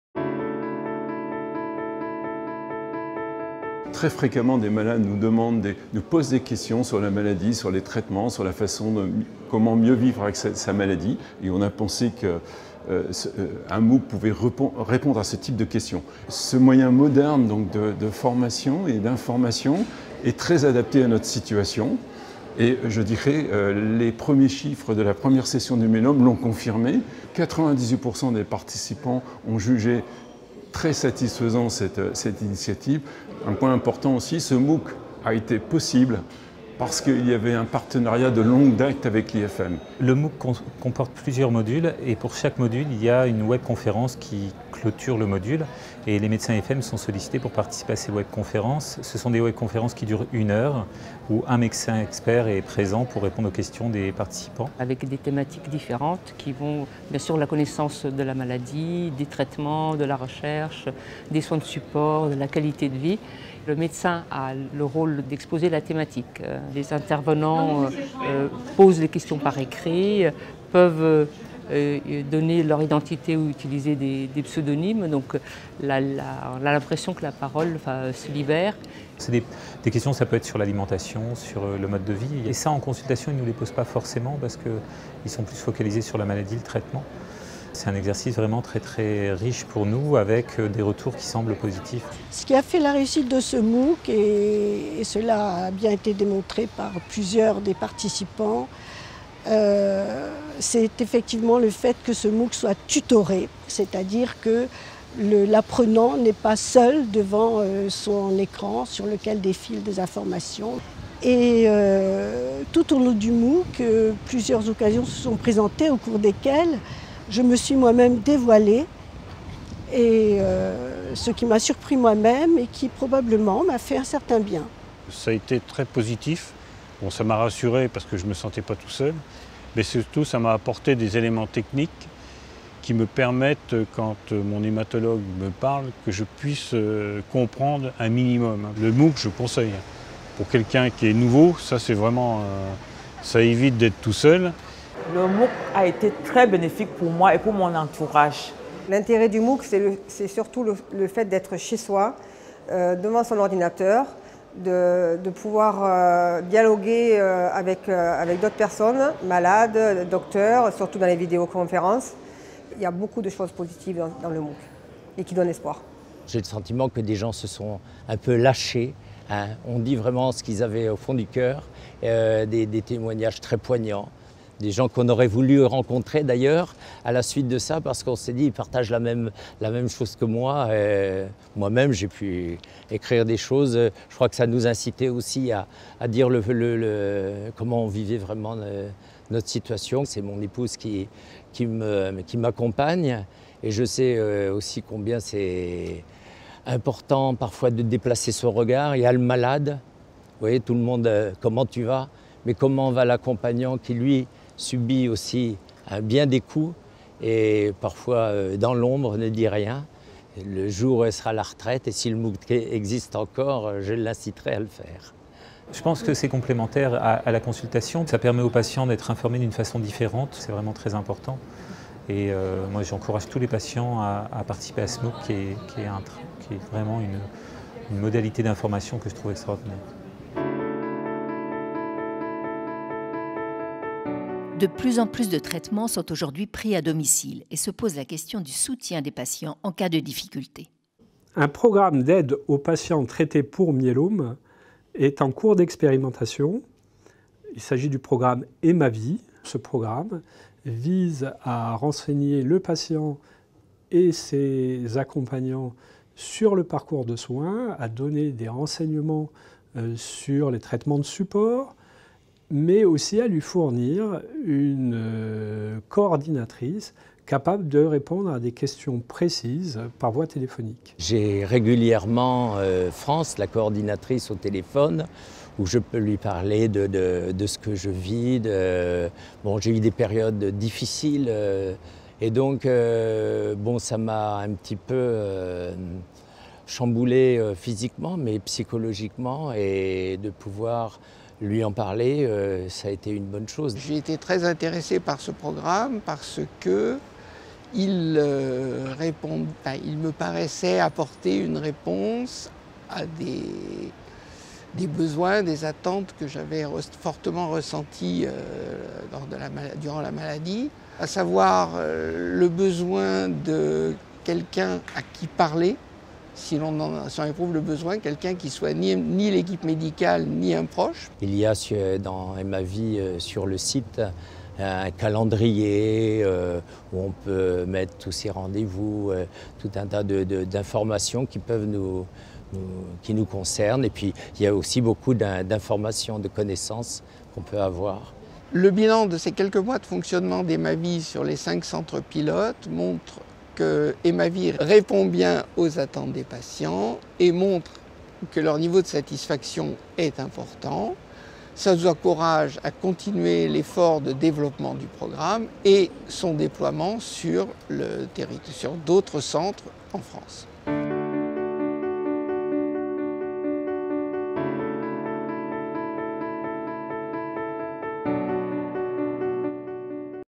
Bande annonce MOOC AF3M
Présentation du parcours pédagogique tutoré de l'AF3M avec Dr SPOC activité de Digital Learning de la société HBMotion